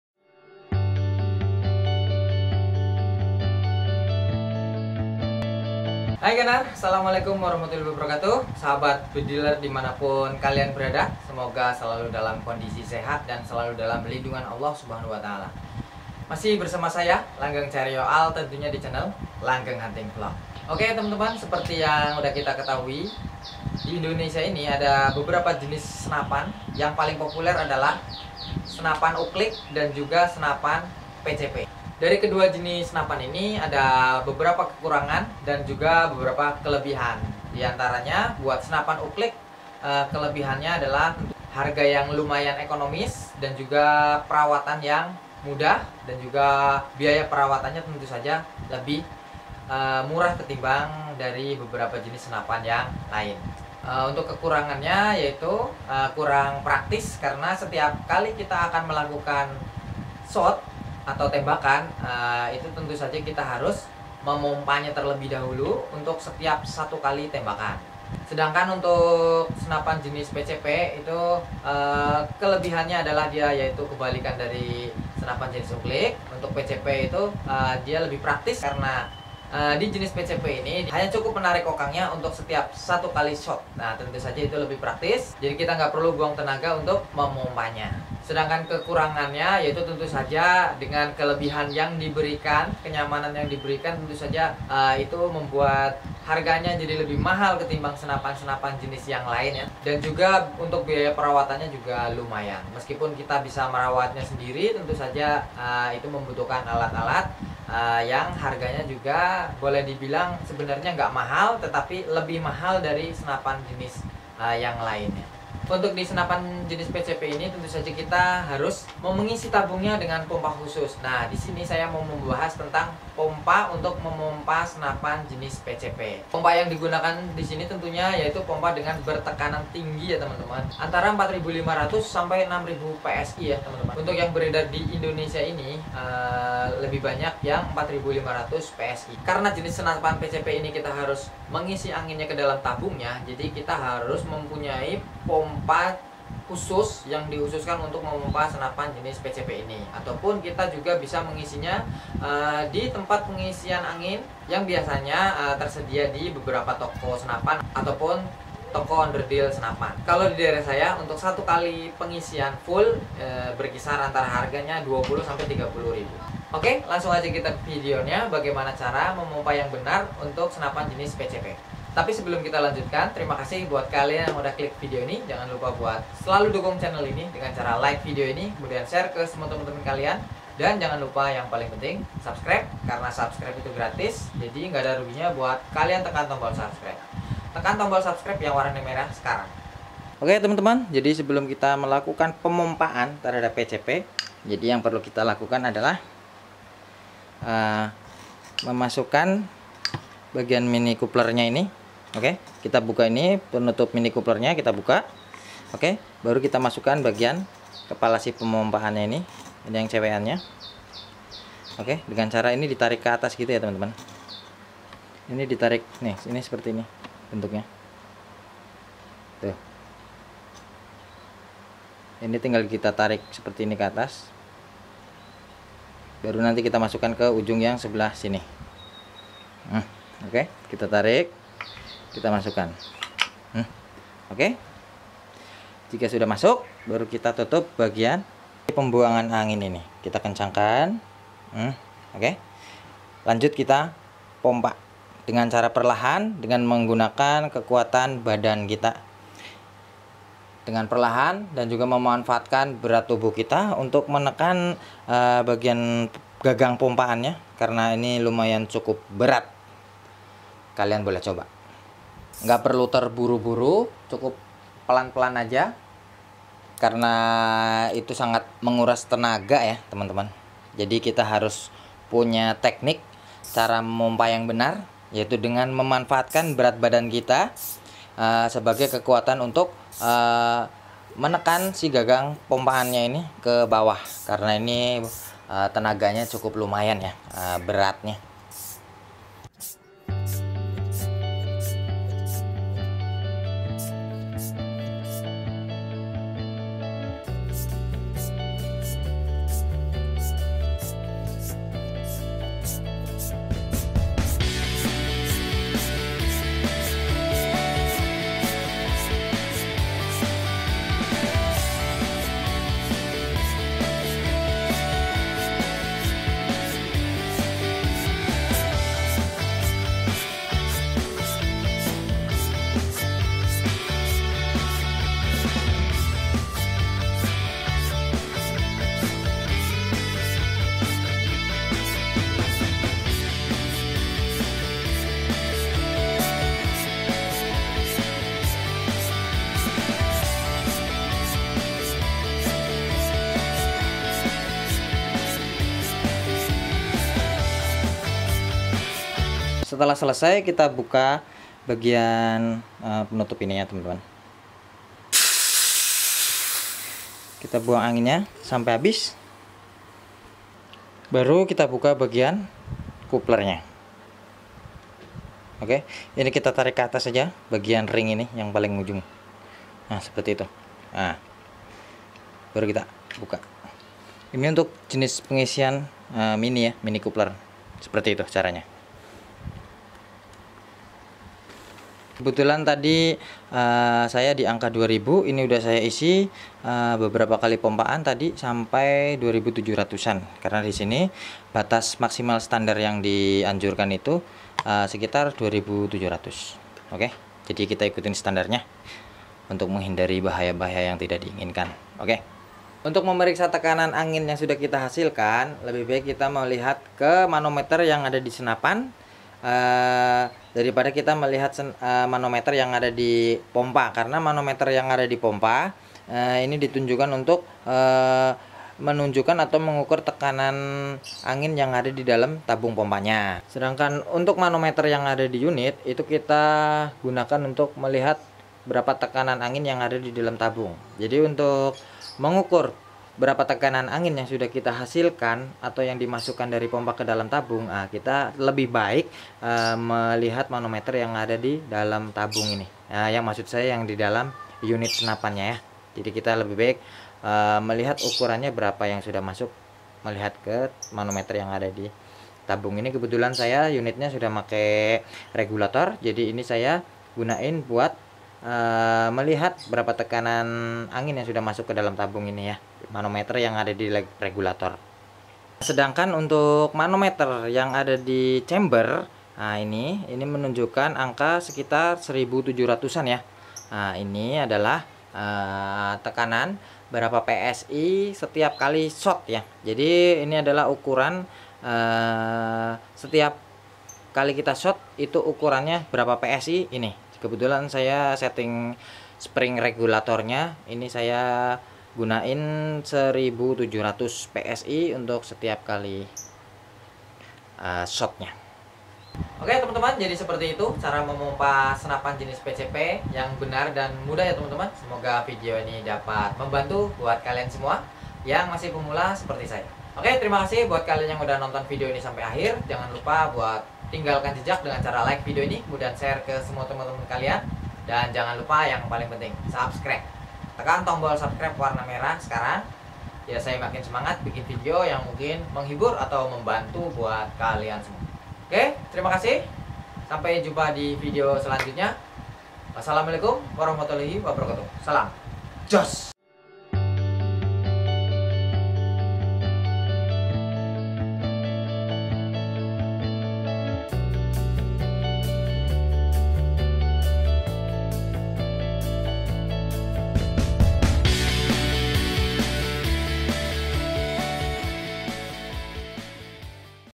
CARA PAKAI POMPA PCP
"Hi Gunner,,
kali ini kita mau berbagi pengalaman tentang cara menggunakan pompa PCP

Seperti apa keseruannya,, makanya tonton video ini sampai habis, dan jangan di SKIP.

JooooOssssS. !